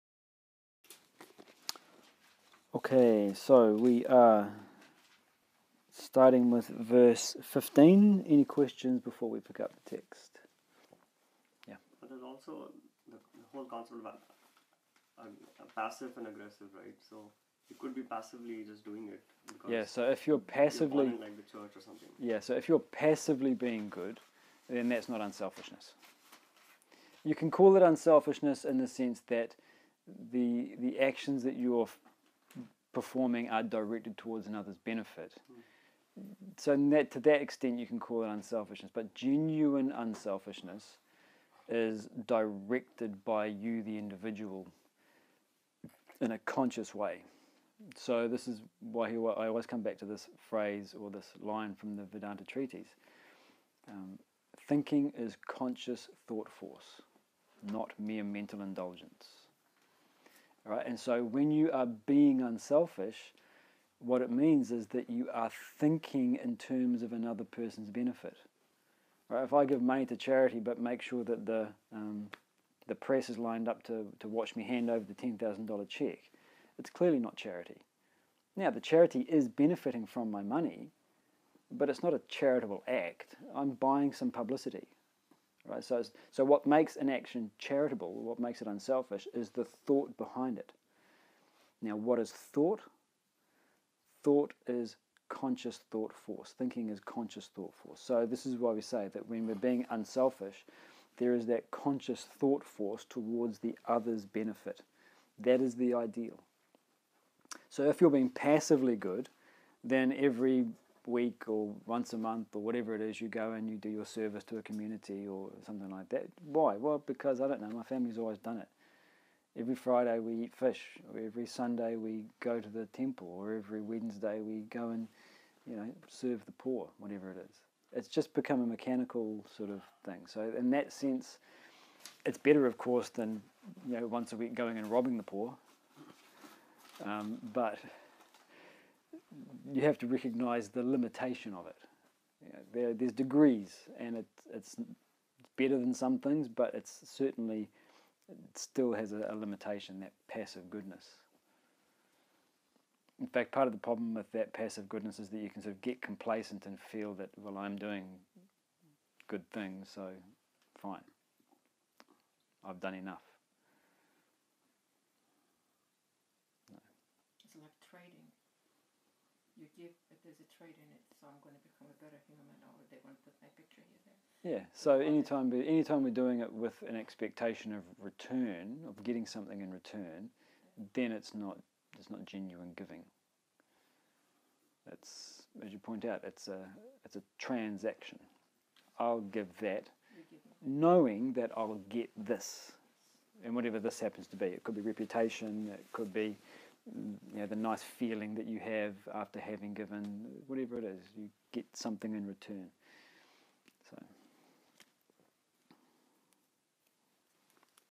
What is Genuine Unselfishness?
Excerpt from a weekly Vedanta study group conducted by Glen Callahan.

Passive unselfishness vs. active unselfishness.